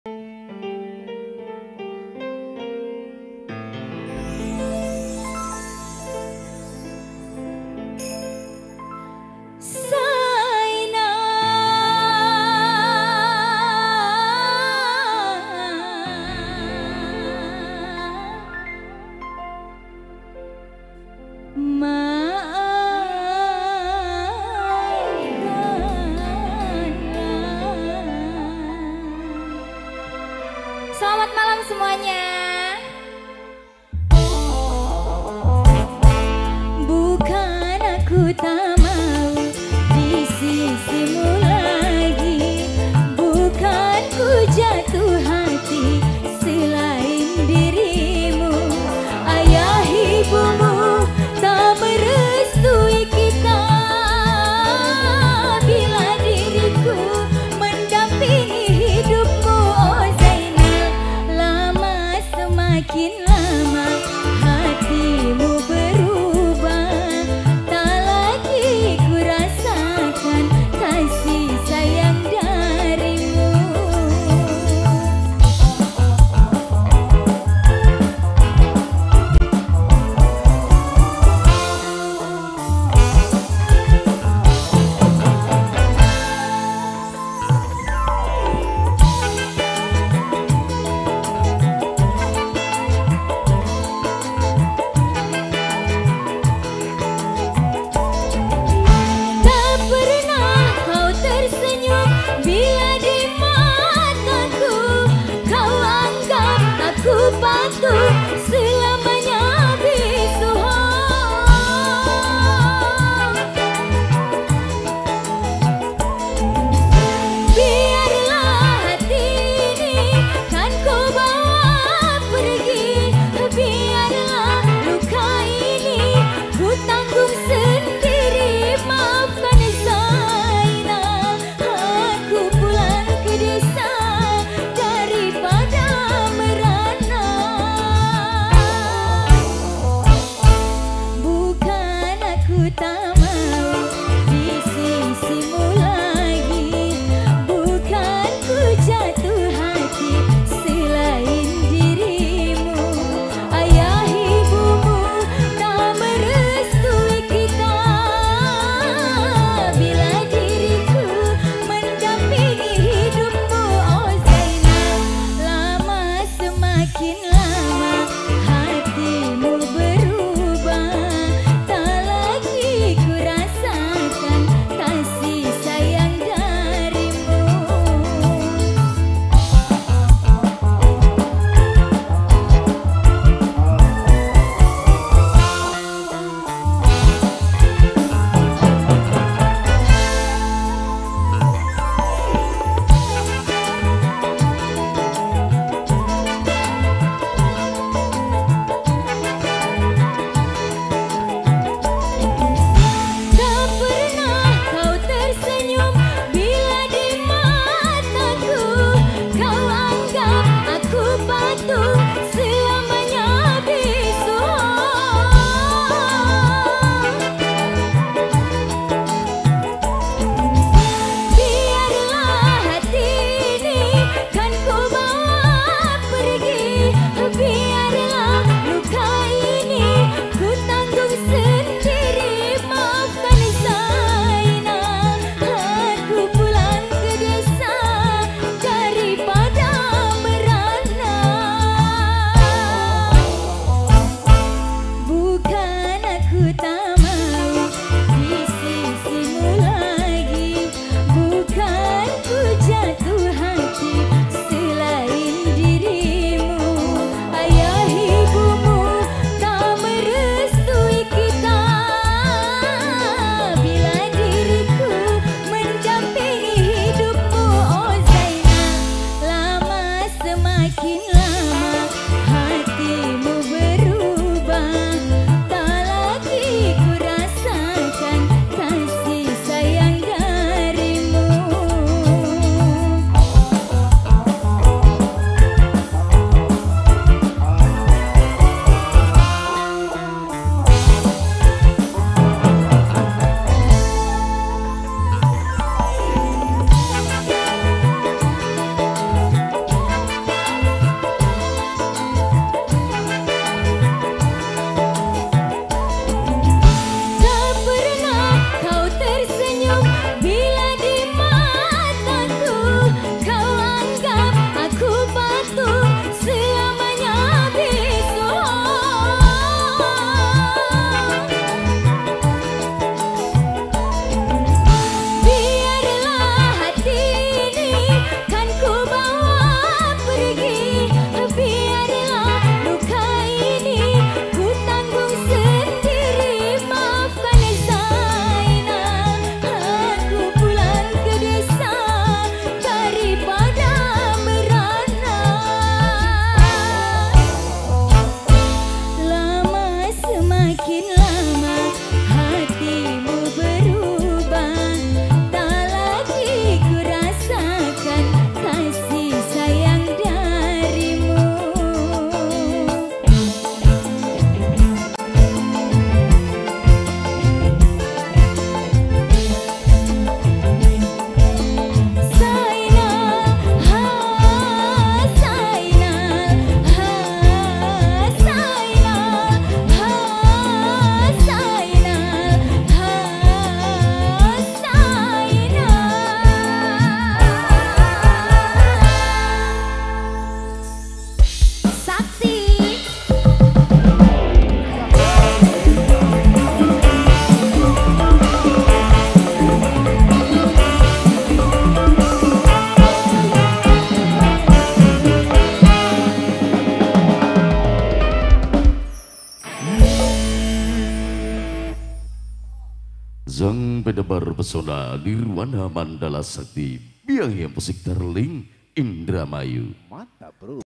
ZAENAL COVER DEDE NELISA SHOW NMS HAJAT BPK.AIPTU WARSA WIJAYA S.H DAN IBU.Dr.FITRIA RAHAYU MM
Dokumentasi Rasulan  Aish Zahida Putri Rahayu
Dokumentasi Khitanan Abhi Satya Putra Wijaya Dan Abhi Sakti Putra Wijaya
Pesta Hajat Kel. Bpk. Aiptu Warsa Wijaya S.H & Ibu dr. Fitria Rahayu MM
Dilaksanakan Pada Hari Jum'at, 18 Oktober 2024
Bertempat Di Jl. Bungkul Barat Desa Bojongsari  Rt.01 Rw.03 Kec. Indramayu Kab. Indramayu - Jawa Barat